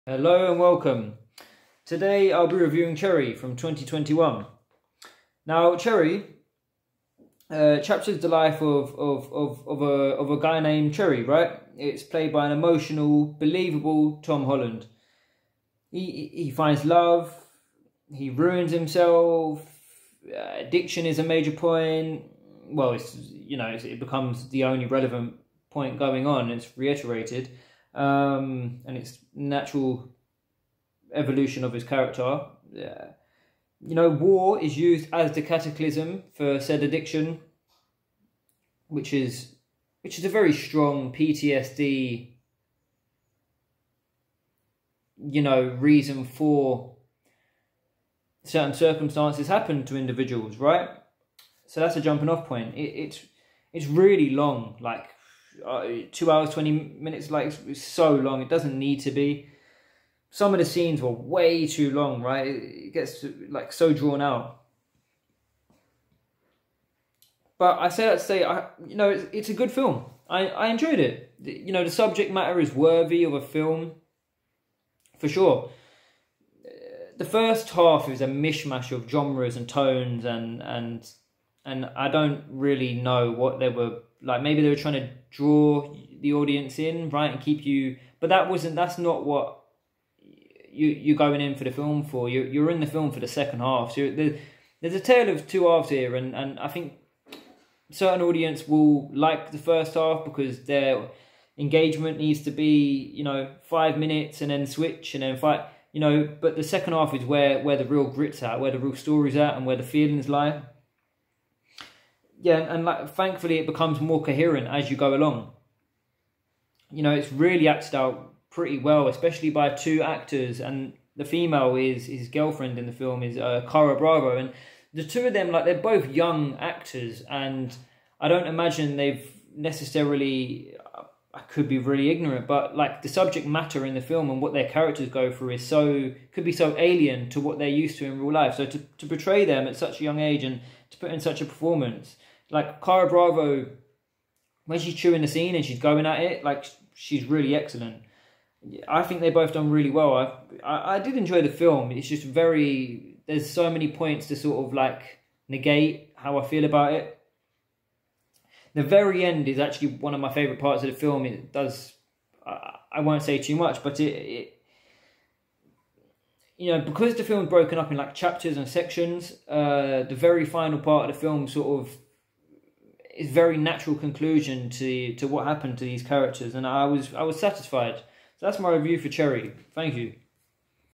CHERRY 2021 Movie Review
welcome, today I will be reviewing
CHERRY 2021

directed by
Anthony & Joe Russo

starring
Tom Holland, Ciara Bravo

All views are solely my own